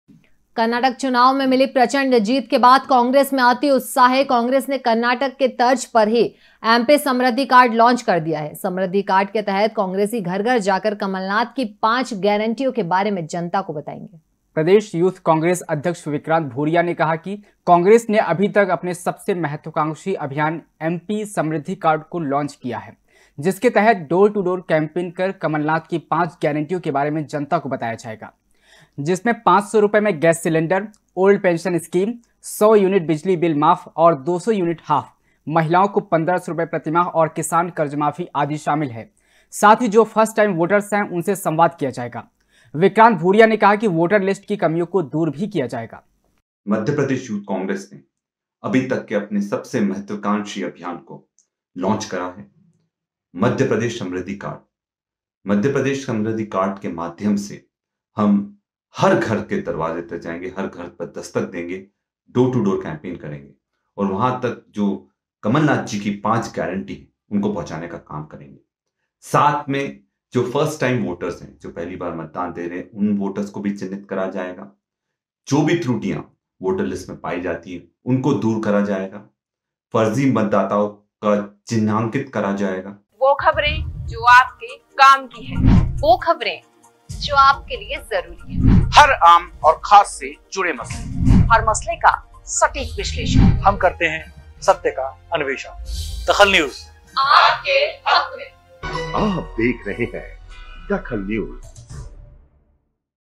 DAKHAL NEWS एमपी में यूथ कांग्रेस का मेगा कैंपेन शुरू
एमपी में यूथ कांग्रेस का मेगा कैंपेन शुरू
जनता को नाथ की गारंटी बताएगी कांग्रेस